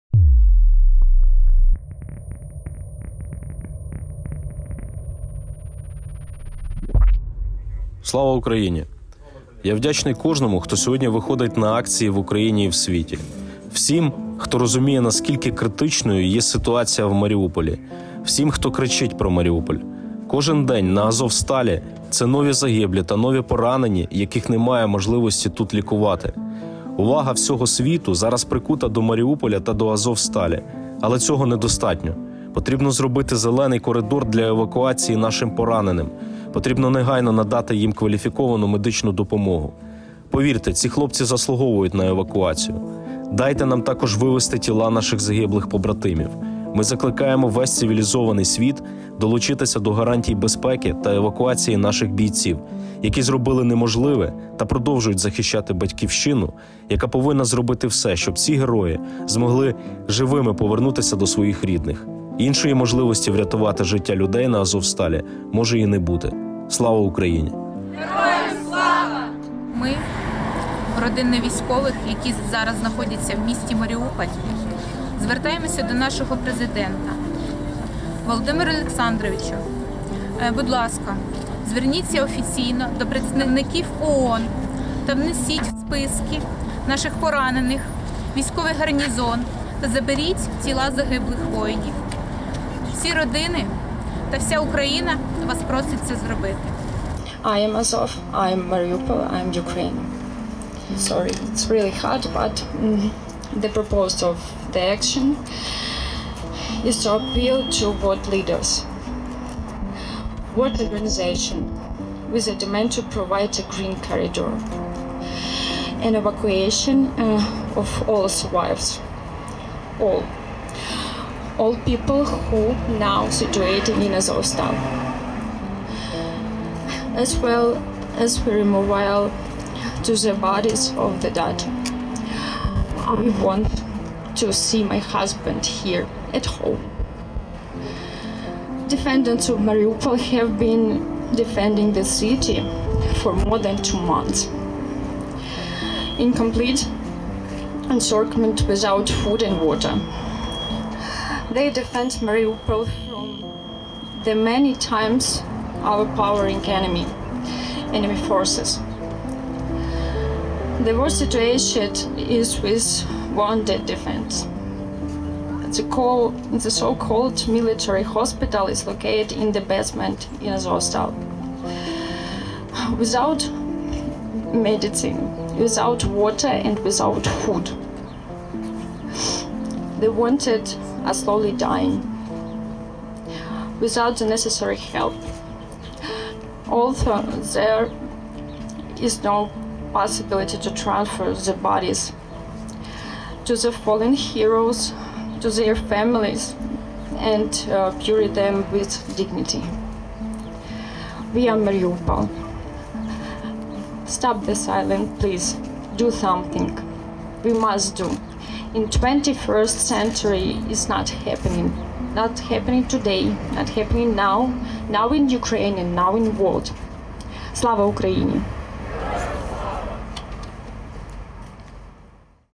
Родичі та близькі "Азову" люди провели акцію у Києві - Врятуйте захисників Маріуполя
"Поки що ніхто не збирається забирати ані поранених, ані військовий гарнізон. Піднімати суспільний резонанс - єдиний вихід. Захисники Маріуполя, які вже зробили неможливе, потребують допомоги українців", - пишуть азовці